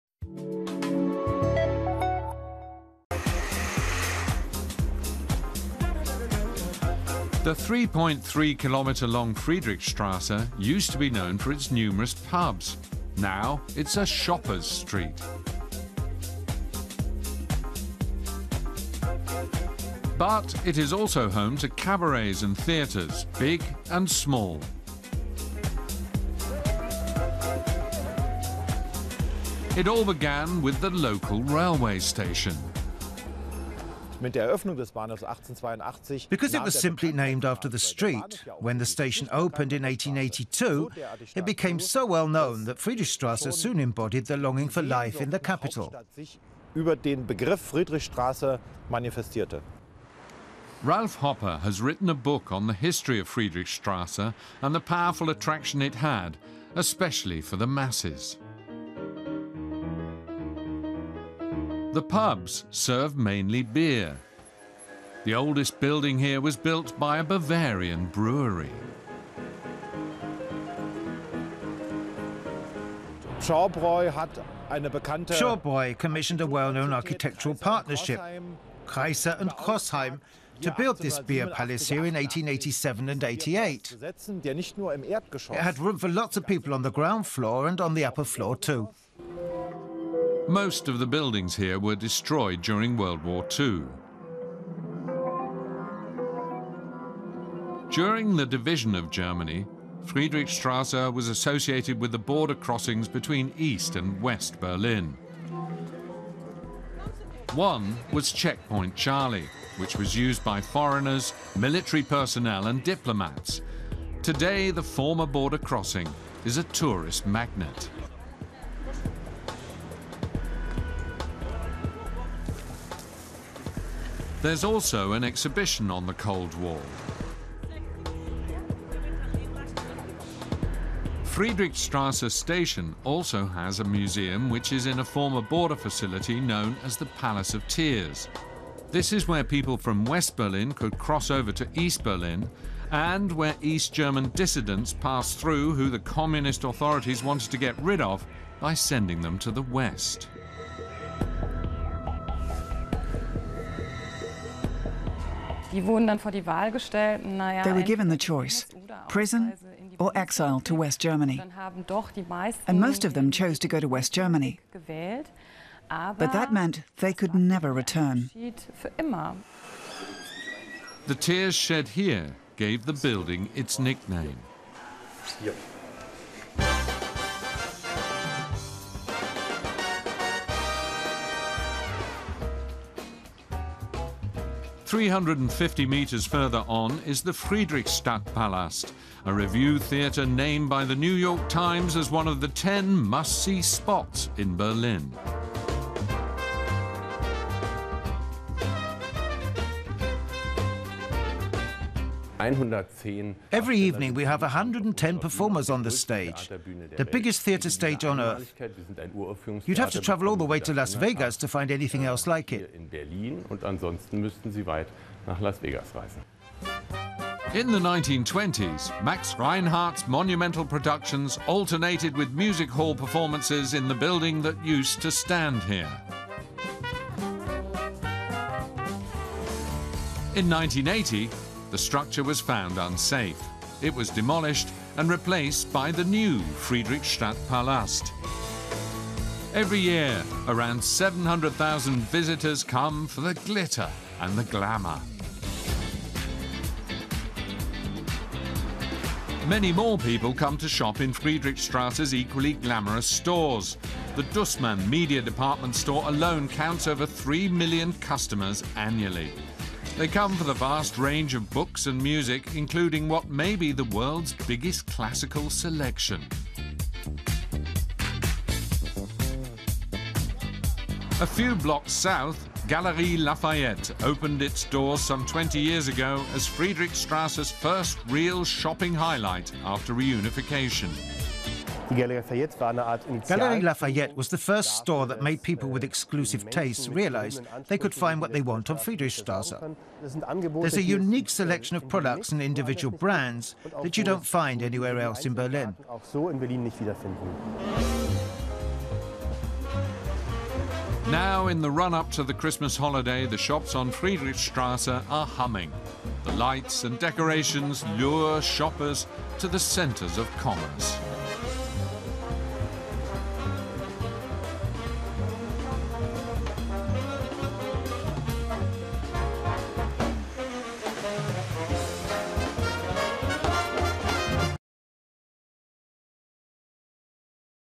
Berlin Friedrichstrasse | Discover Germany
You can find out about German history on Berlin's Friedrichstrasse, enjoy the nightlife and visit luxury stores. Berlin's Friedrichstrasse is one of the main streets in the heart of the city. It is around 3 kilometers long and it is a great place to go shopping if you have plenty of cash to spare.